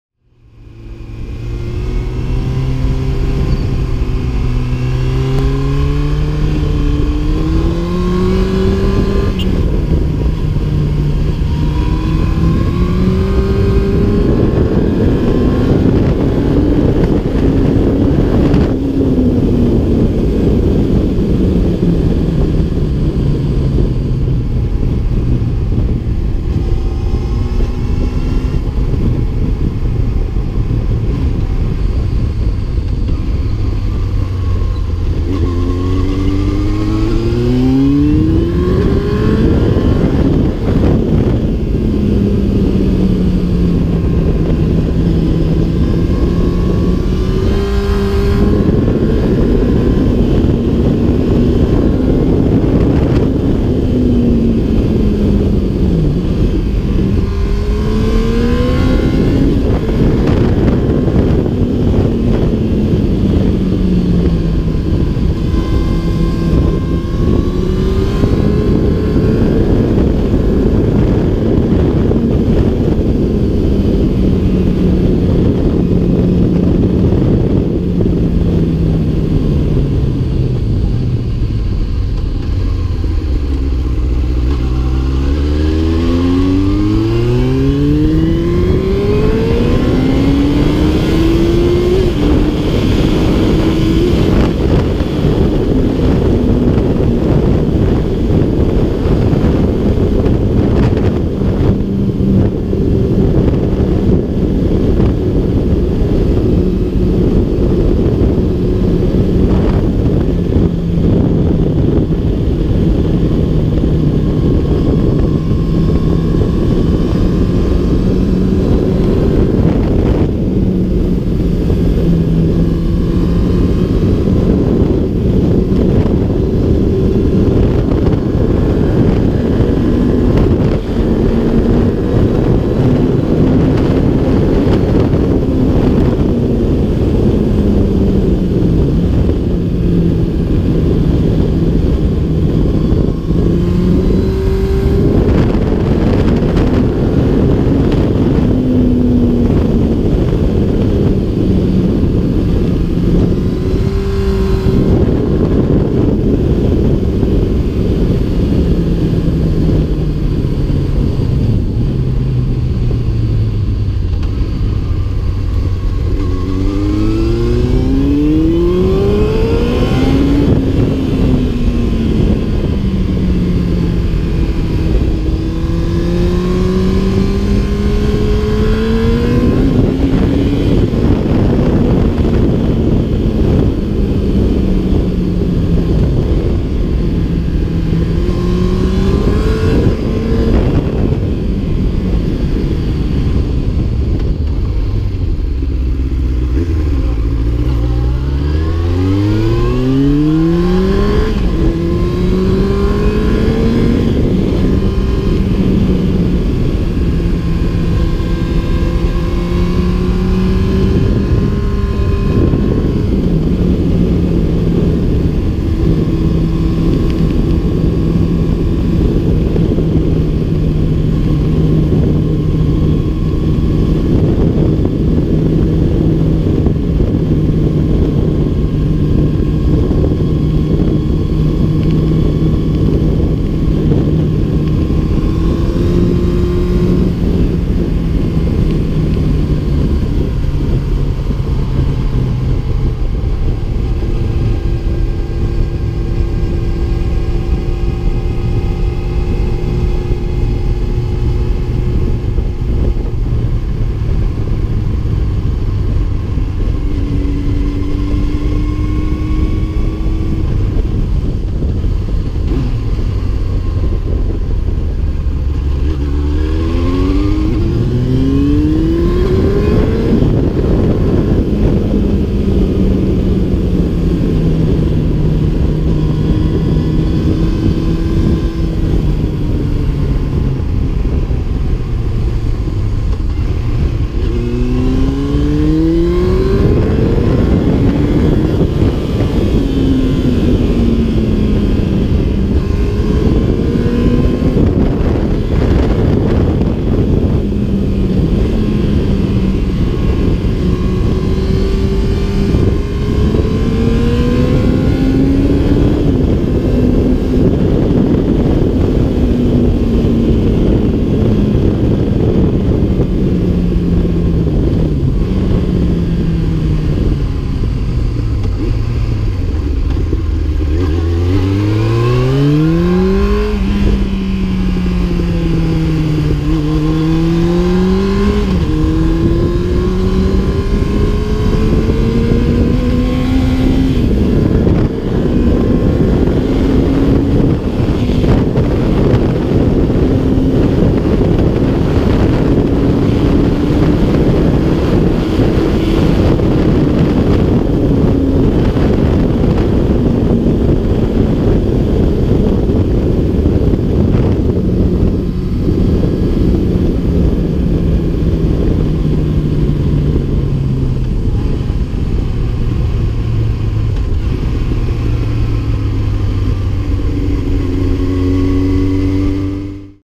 R1 gopro test
A ride on my 2001 Yamaha R1